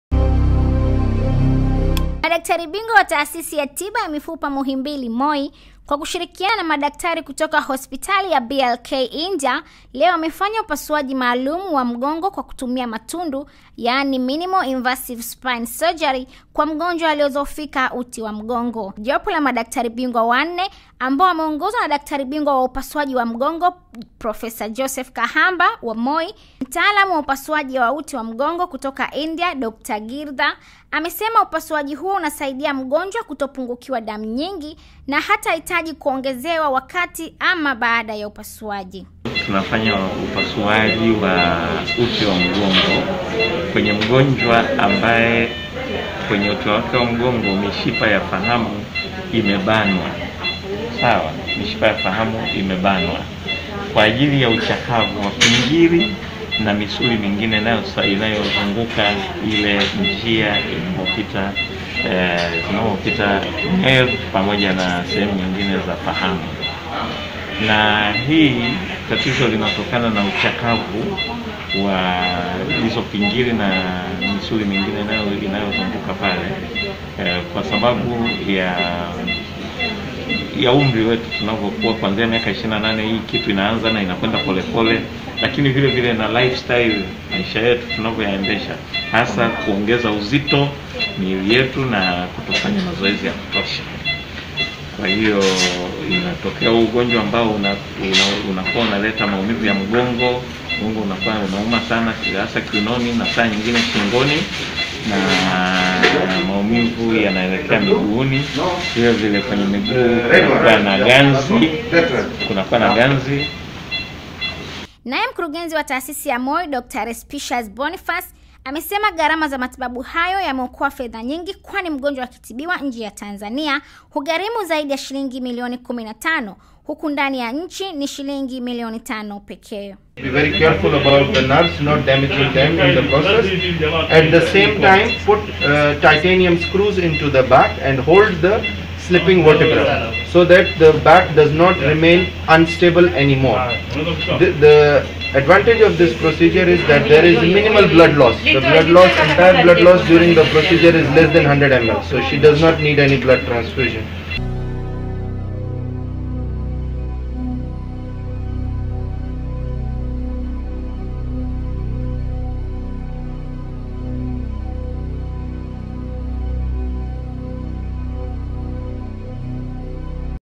MUHIMBILI WAFANYA UPASUAJI WA MGONGO KWA KUTUMIA MATUNDU
Madaktari Bingwa wa Taasisi ya Tiba Mifupa Muhimbili (MOI) kwa kushirikiana na madaktari hospitali ya BLK India leo wamefanya upasuaji maalum wa mgongo kwa kutumia matundu (Minimal Invasive Spine surgery) kwa mgonjwa aliyedhoofika uti wa mgongo.